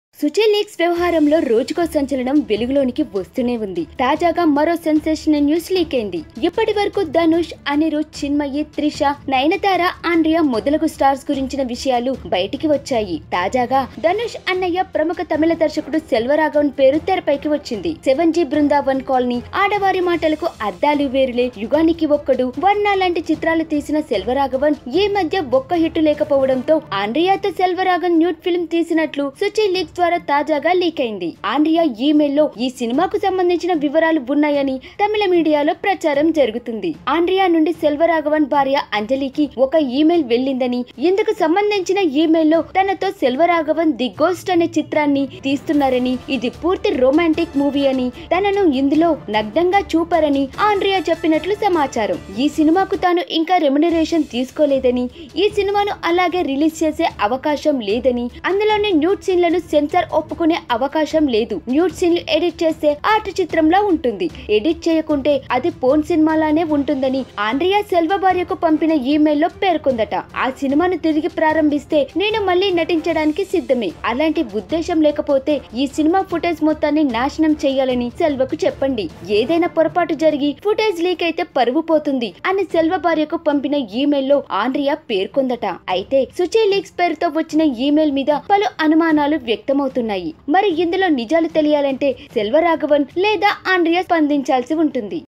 Dhanush Brother Directs A Nude Film With Suchi Leak Heroine || K News
Watch Dhanush Brother Directs A Nude Film With Suchi Leak Heroine